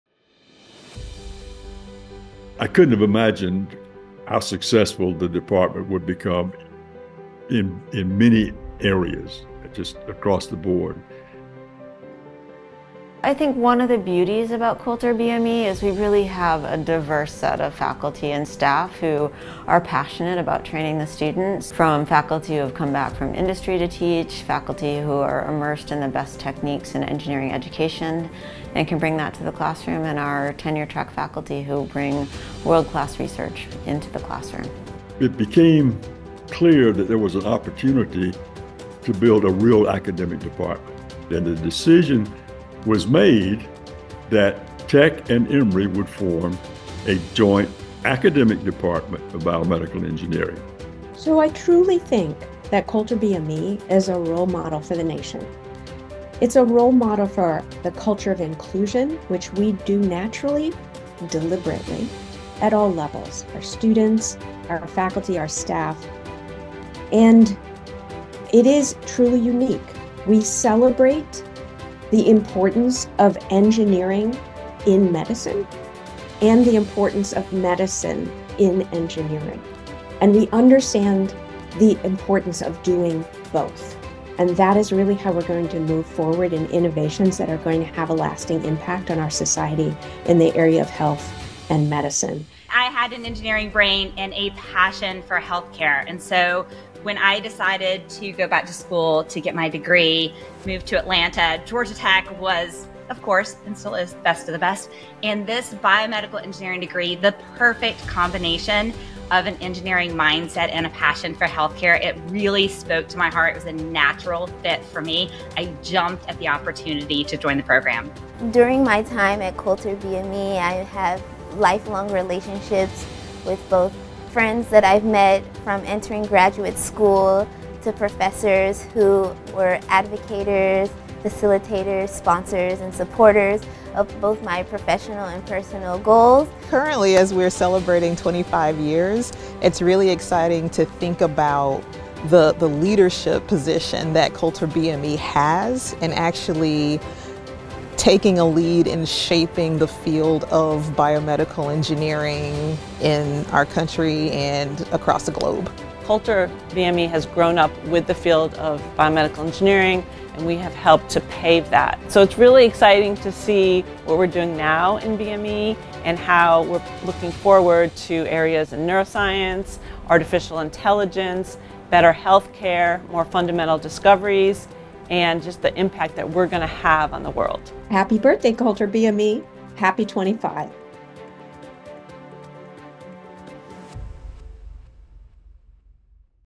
Wallace H. Coulter Department of Biomedical Engineering 25th Anniversary
Since our first students arrived in 2000, the Wallace H. Coulter Department of Biomedical Engineering has been committed to improving the health and well-being of others by fostering the next generation of leaders in the biomedical field. This year, we celebrate those efforts and our continued progress as a transformative leader in solving unmet clinical needs.